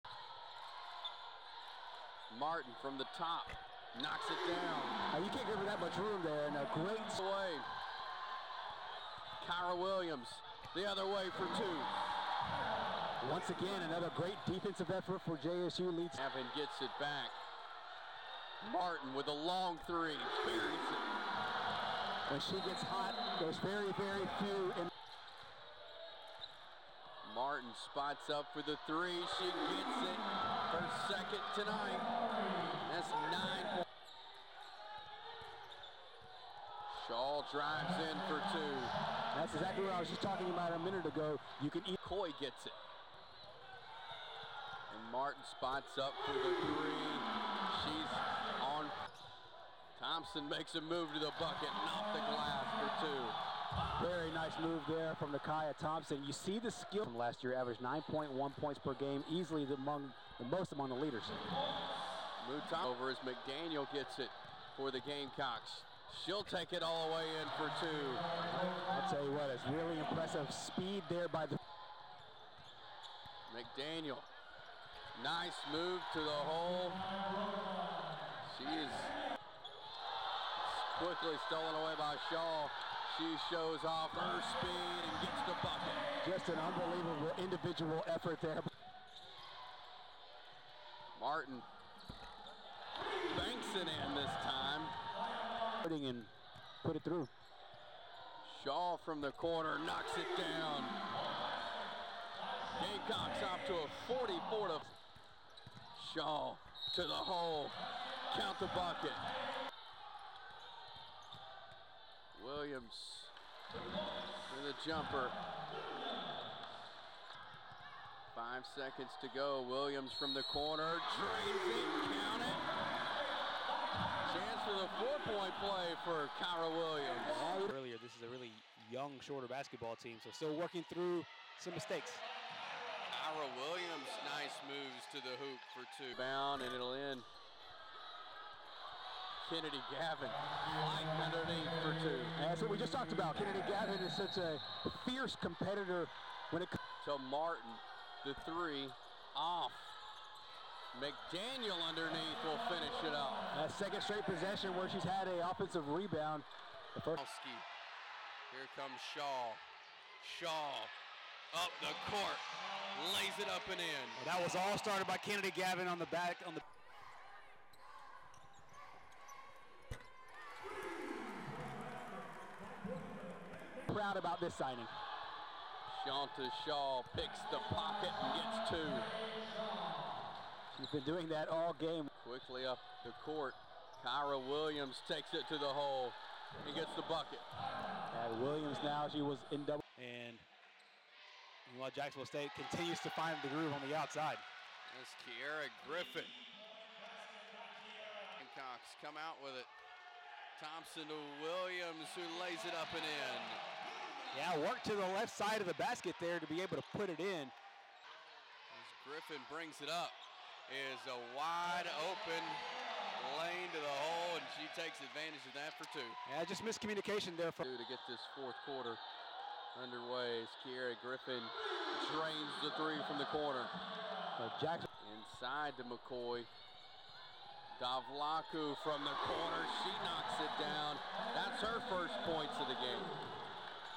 JSU Women's Basketball vs Shorter Highlights November 7th, 2022
Highlights from the JSU Women's basketball 114-33 win against Shorter at Pete Matthews Coliseum in Jacksonville Alabama.